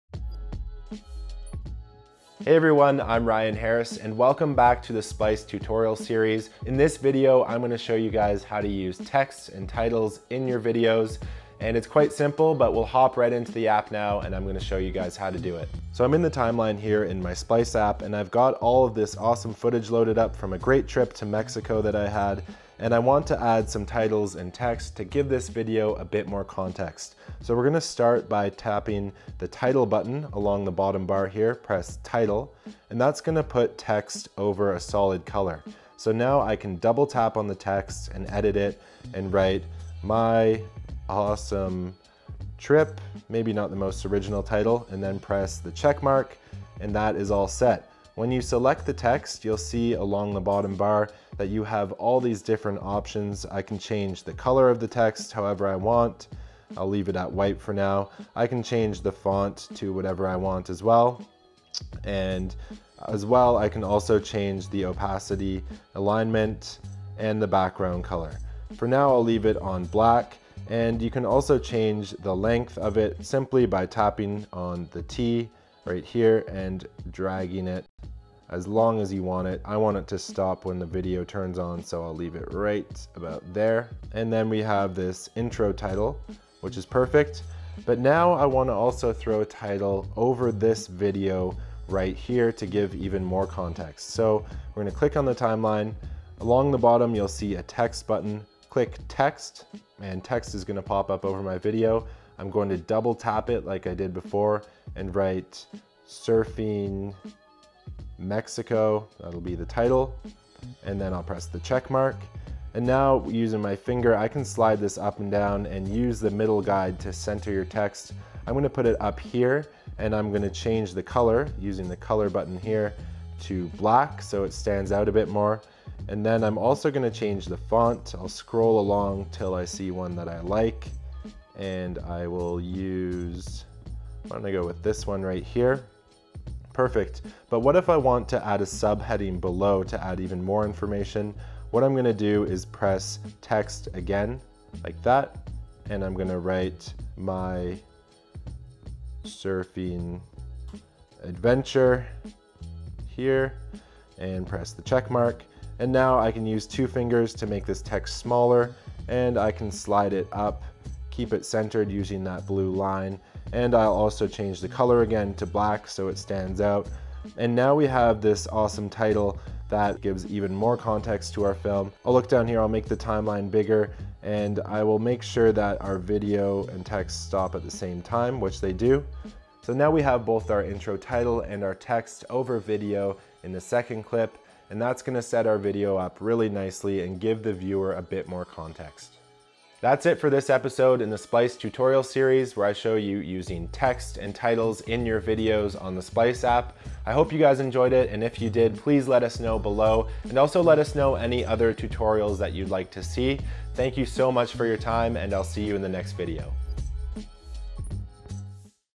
How to add Text and Titles to your videos
Hey, Splicers? Welcome back to the Splice Basics Tutorial Series! Episode 2. 🎬
Ryan Harris here. Learn how to play with text and titles to level up your videos. We promise—it's simple!

Video Sections:
0:00 Intro
0:14 How to add a title to your video
1:27 How to add text on your clips
2:17 Adding multiple texts to your video
3:10 Outro

"The Best Video Editor for iPhone"
- Lifehacker